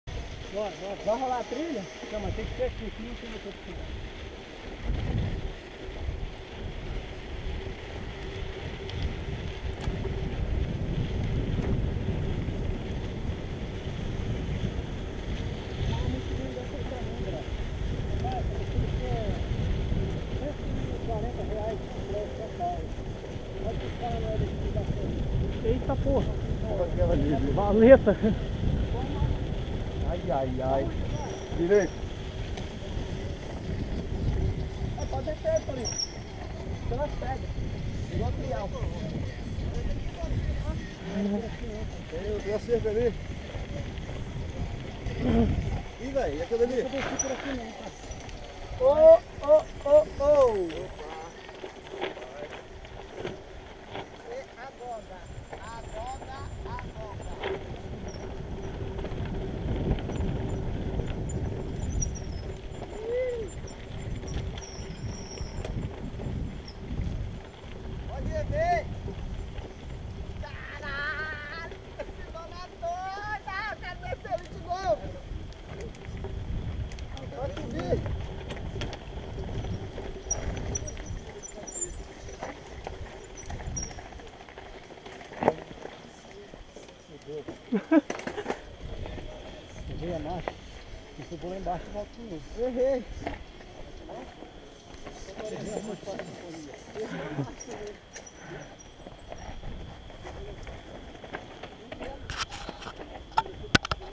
MTB, Gasoduto Jacaraipe, Serra-ES parte 4 Filho chora e a mãe não ouve
Registro da nossa passagem pela região do gasoduto, localizado entre Nova Almeida e Jacaraipe na Serra, região com várias trilhas de todos os níveis de dificuldades.